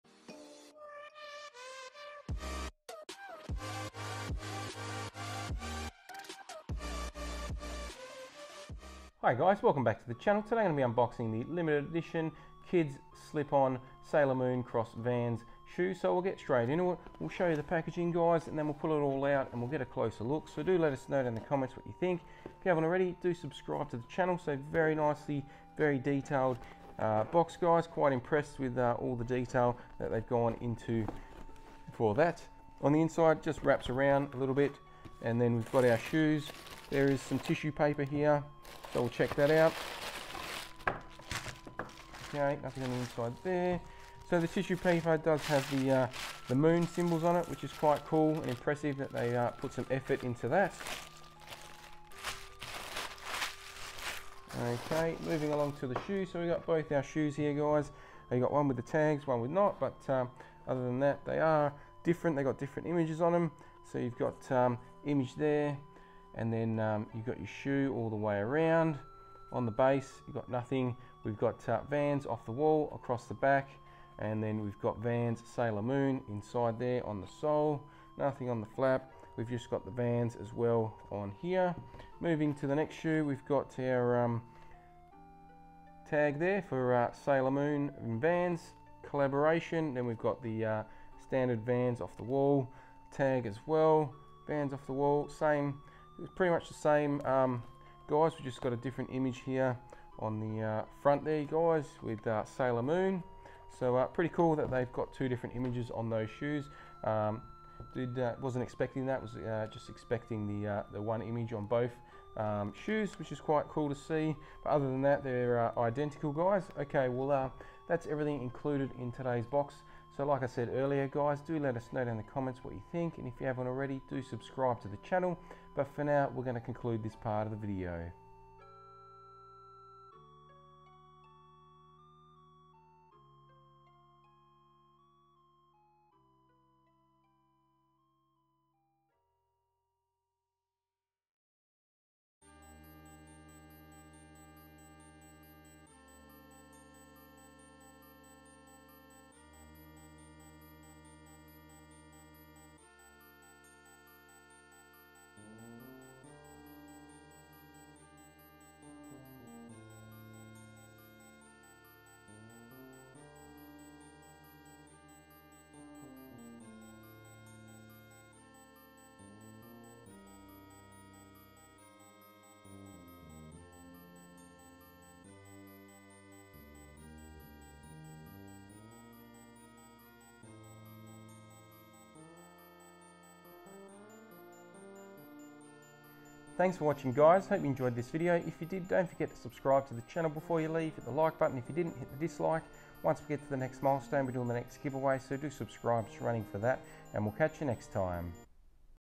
Ep 1563 - Limited Edition Kids Classic Slip-On Vans X Sailor Moon Shoes Unboxing
Hi Guys

Welcome to the channel.  Please enjoy my unboxing video of the Vans X Sailor Moon Shoes.

Fan Mail -

Australia 6906